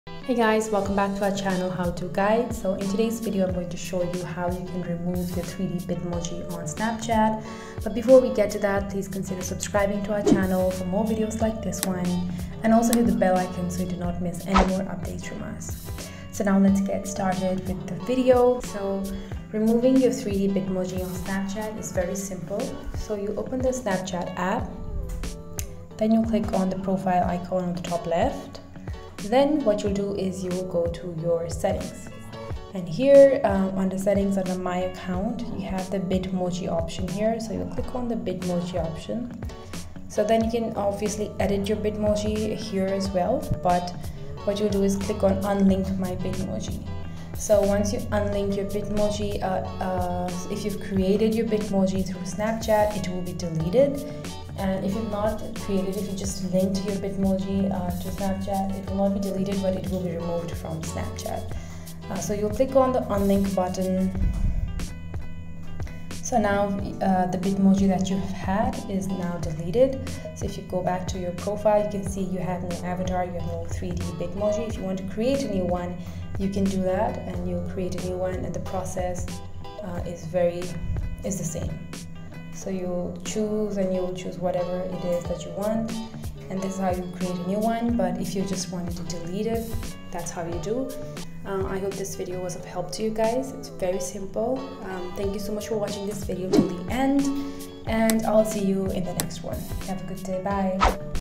How To Remove 3D Bitmoji On Snapchat (2022)
Confused about how to remove 3d bitmoji on snapchat on iphone? This video explains the exact steps on how to remove 3d bitmoji on snapchat. Make sure you watch the video till the end to learn it in much detail.